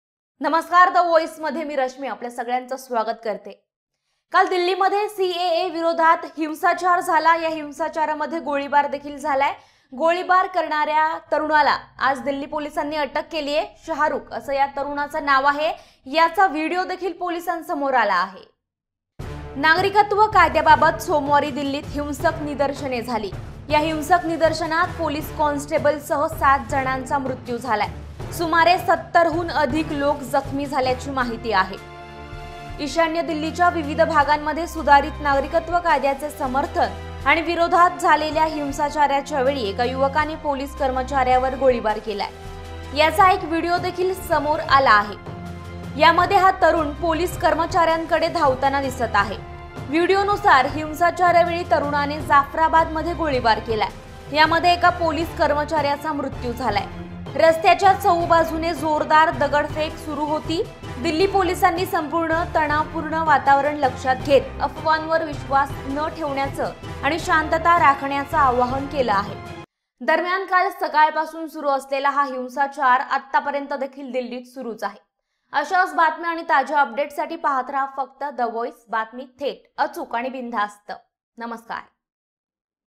Delhi Violence | हिंसाचारात पोलिसांवर गोळीबार करणाऱ्या तरुणाला अटक | The Voice News Marathi
दिल्ली पोलिसांनी CAA विरोधात झालेल्या हिंचारामध्ये गोळीबार करणाऱ्या तरुणाला ताब्यात घेतलं आहे. शाहरुख असं बंदूक घेऊन गोळ्या झाडणाऱ्या तरुणाचं नाव असल्याची माहिती पोलिसांकडून देण्यात आली आहे.

या तरुणाने ईशान्य दिल्लीमध्ये उफाळलेल्या हिंसाचारामध्ये जमावावर गोळीबार केला तर सध्या या पोलिसांच्या ताब्यात आहे.
नागरिकत्व कायद्याबाबत सोमवारी दिल्लीत हिंसक निदर्शने झाली. या हिंसक निदर्शनात पोलिस कॉन्स्टेबलसह 7 जणांचा मृत्यू झाला तर सुमारे 70 हून अधिक लोक जखमी असल्याची माहिती आहे. ईशान्य दिल्लीच्या विविध भागांत सुधारित नागरिकत्व कायद्याचे  समर्थन आणि विरोधात झालेल्या हिंसाचाराच्या वेळी एका युवकाने पोलिस कर्मचाऱ्यावर गोळीबार केला. याचा एक व्हिडिओदेखील समोर आला आहे. यामध्ये हा तरुण पोलीस कर्मचाऱ्याकडे धावत असताना दिसत आहे.
  व्हिडिओनुसार, हिंसाचारावेळी तरुणाने जाफराबादमध्ये गोळीबार केला. यामध्ये एका पोलीस कर्मचाऱ्याचा मृत्यू झाला आहे. रस्त्याच्या चहुबाजूने जोरदार दगडफेक सुरू होती. दिल्ली पोलिसांनी संपूर्ण तणावपूर्ण वातावरण लक्षात घेत अफवांवर विश्वास न ठेवण्याचे आणि शांतता राखण्याचे आवाहन केले आहे.
  दरम्यान, काल सकाळपासून सुरू असलेला हा हिंसाचार आतापर्यंत सुरू आहे. आज सकाळच्या सुमारास ब्रम्हापूरी आणि मौजपूर इथे दोन गटांमध्ये दगडफेक करण्यात आली आहे.